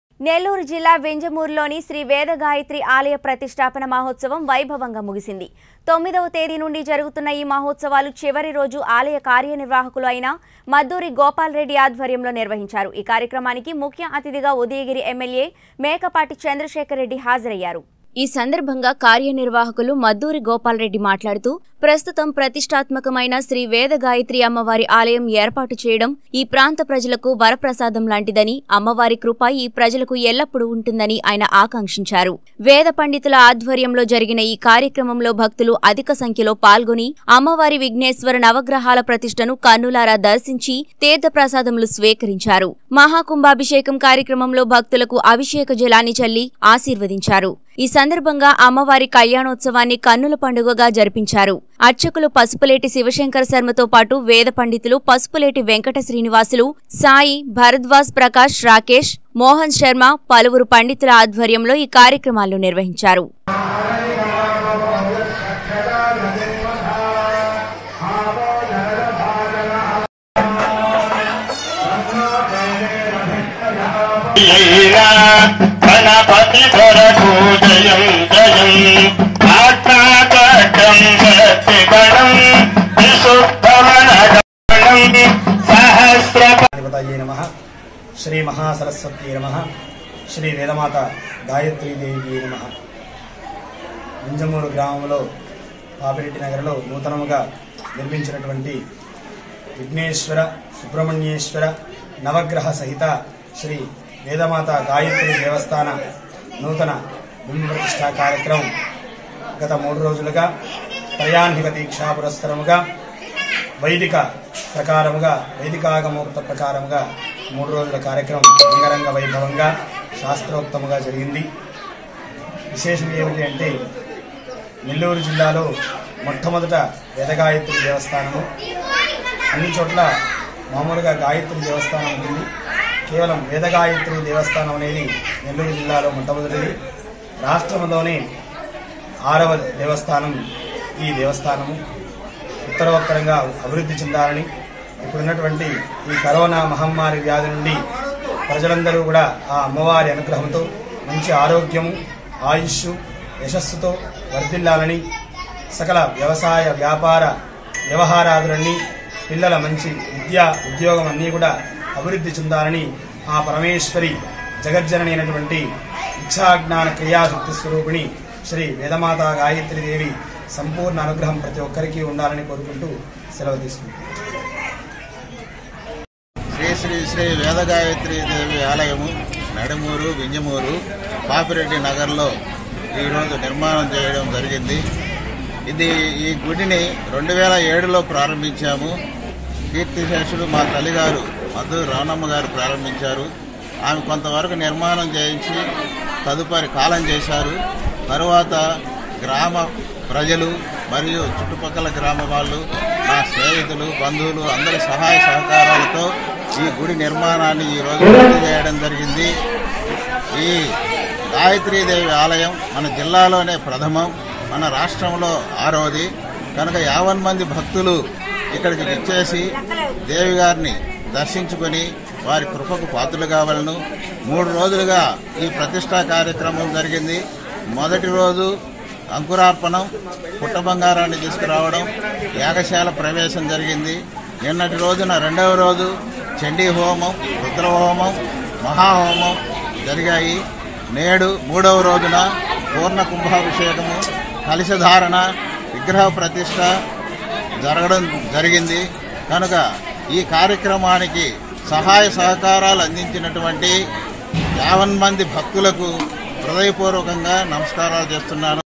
చివరిరోజు వైభవంగా ముగిసిన శ్రీ వేధగాయత్రి ఆలయ ప్రతిష్ఠాపన మహోత్సవం - నెల్లూరు జిల్లా
చివరిరోజు వైభవంగా ముగిసిన శ్రీ వేధగాయత్రి ఆలయ ప్రతిష్ఠాపన మహోత్సవం - నెల్లూరు జిల్లా | 19 Newstelugu